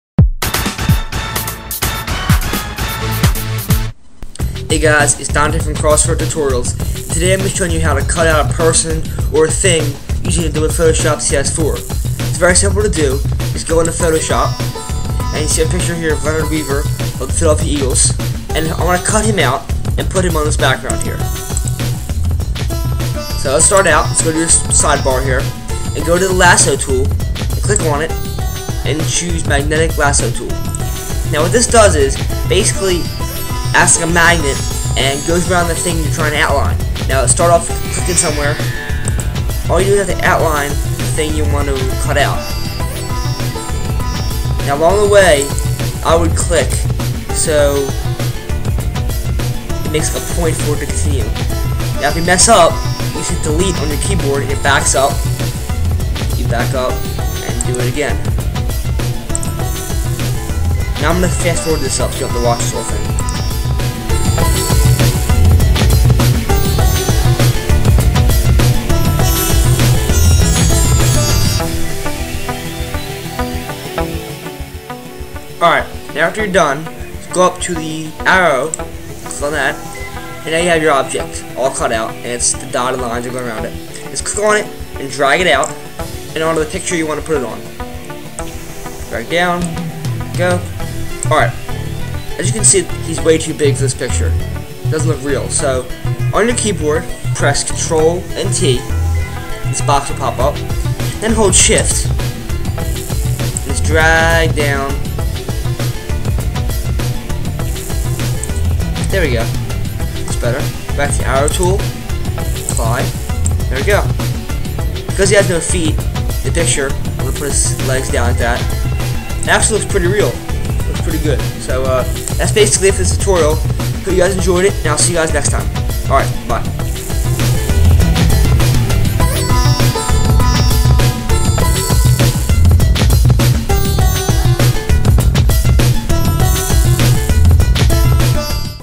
How to cut out a person or object in Adobe Photoshop CS4
In this video I show you how to cut someone or something out of a picture using the magnetic lasso tool and placing it in another picture.